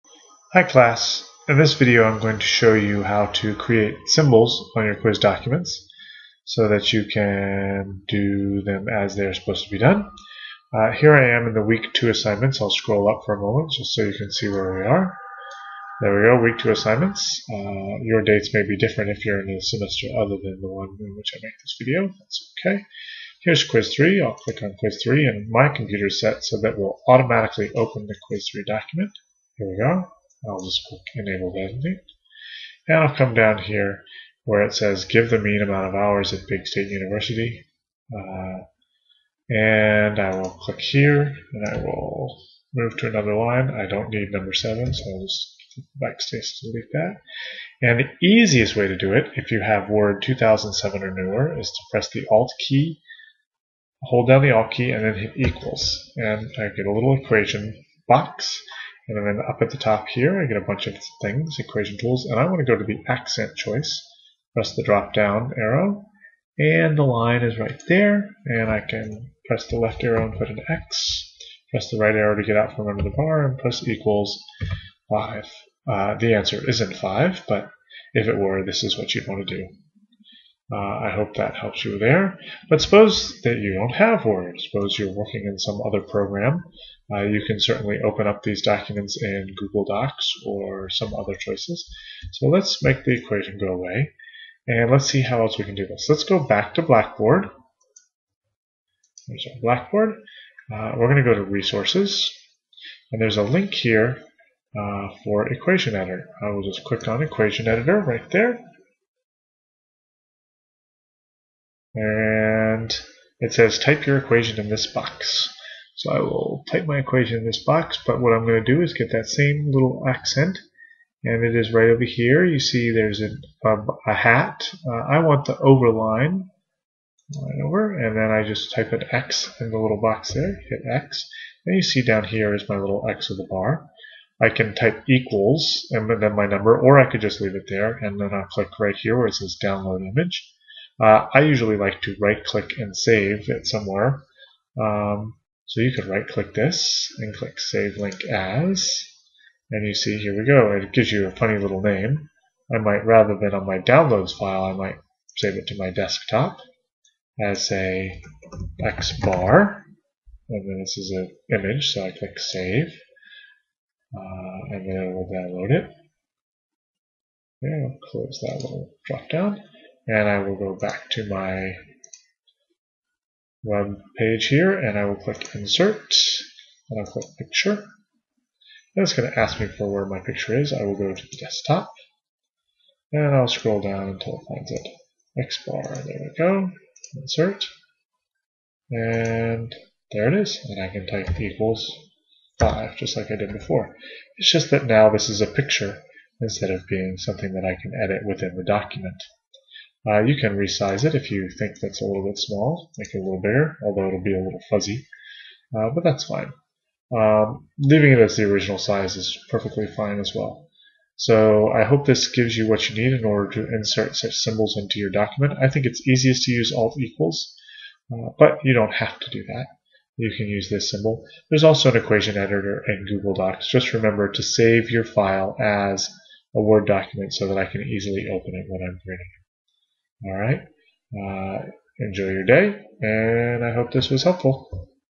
Creating Symbols
Learn how to insert symbols, like x-bar, into Microsoft Word documents.